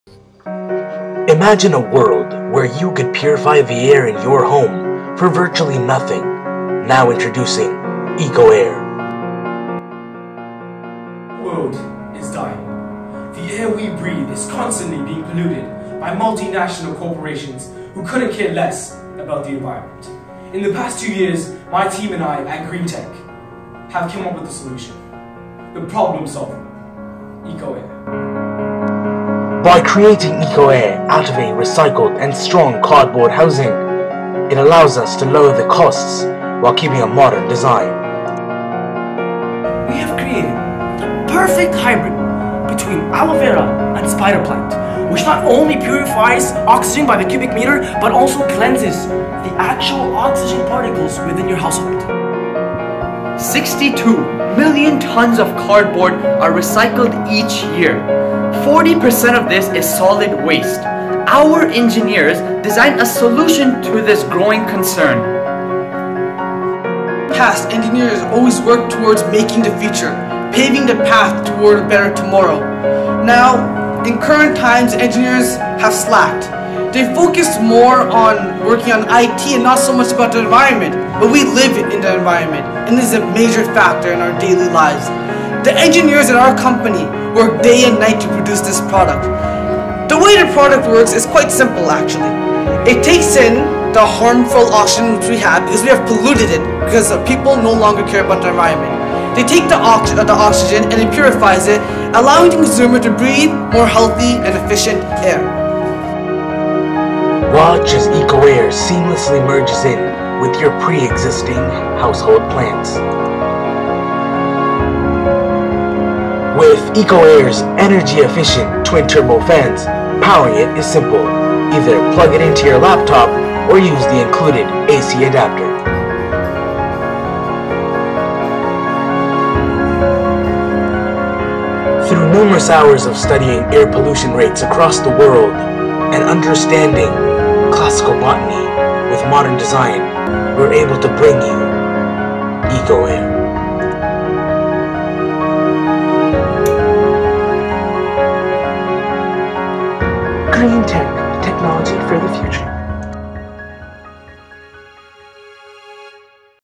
DECA Idea Challenge 2015 - Eco Air
Eco Air is a product created by GreenTech. Its primary job is to purify air in your home. By using recycled cardboard as the main element of design , we are able to not only save the environment but also costs. The concept is quite simple, air is sucked in , then plants purify their, and it is finally blown back out.